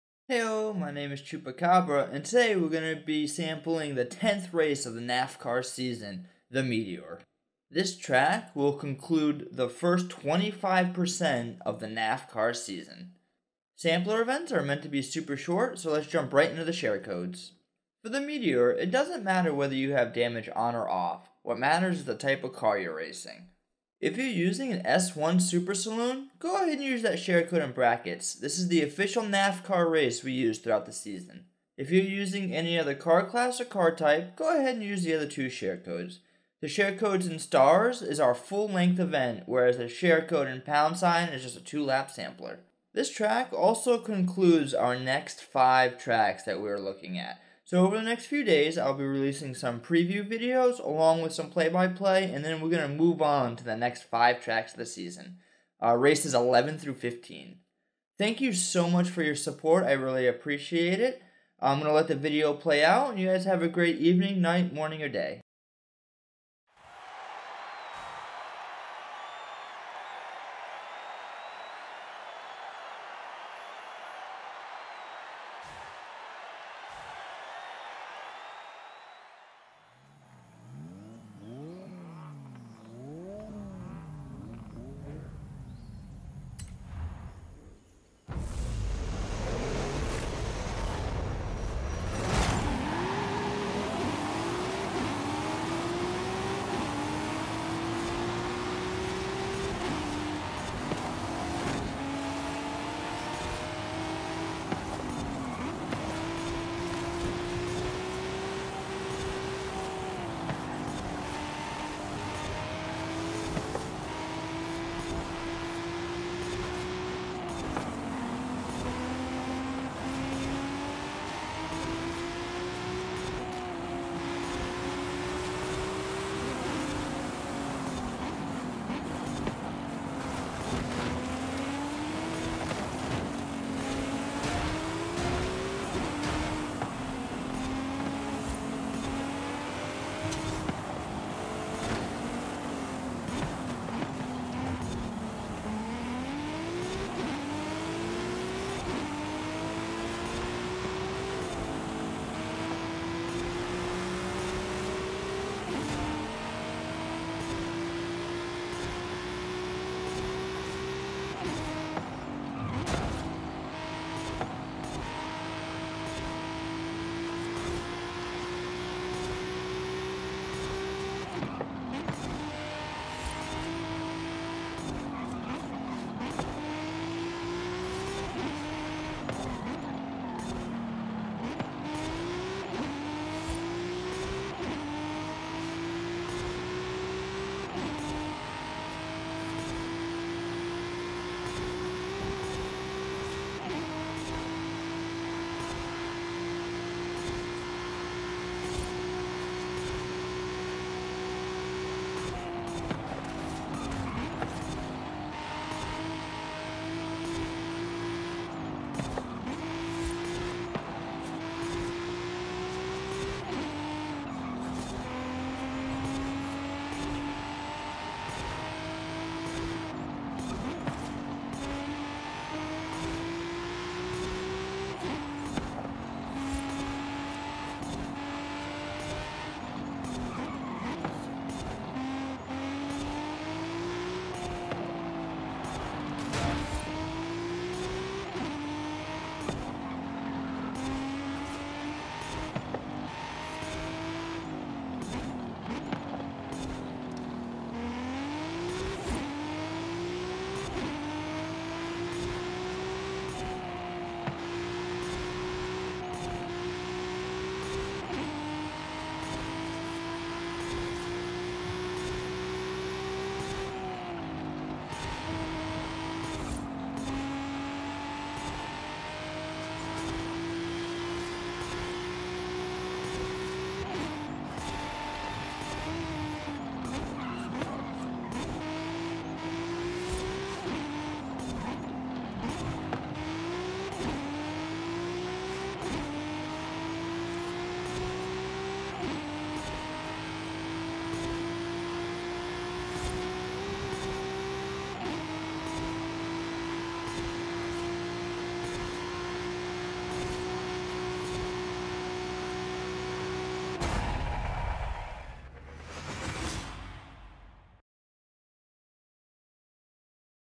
NAFCAR# 10/40: The Meteor- Sampler
Heyoo! Double upload today! This is our sampler event for The Meteor. This is a 100% paved road race! I will have preview + Play by Play videos of this track hopefully this week! Thank you so much for your support!!!

SHARE CODES:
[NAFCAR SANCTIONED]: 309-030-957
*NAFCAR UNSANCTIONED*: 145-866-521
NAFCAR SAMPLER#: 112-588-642